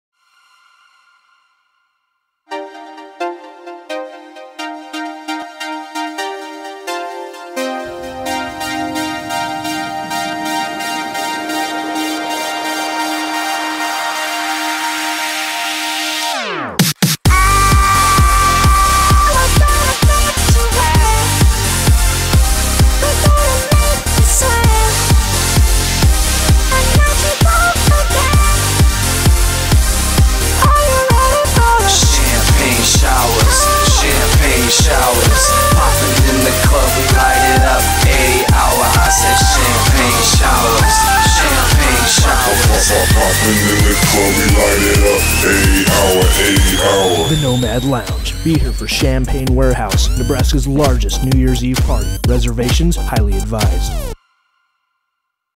Champagne Warehouse // New Year's 2012 // NOMADNYE.COM
The Night Light Group presents Champagne Warehouse: Nebraska's Largest NYE Party at Nomad Lounge Saturday evening December 31st, 2011.

- FREE CHAMPAGNE
- INVITED CELEBRITY GUESTS
- ICE SCULPTURES
- WHITE CHOCOLATE FOUNTAIN
- BALLOON DROP
- DANCE MUSIC

9PM - 2AM │ Dress code is enforced │ Management reserves all rights.
Nomad Lounge │ 10th & Jones │ Omaha, NE

1-888-71-TICKETS

*Limited VIP tickets & Platinum seating available. Reservations are highly advised.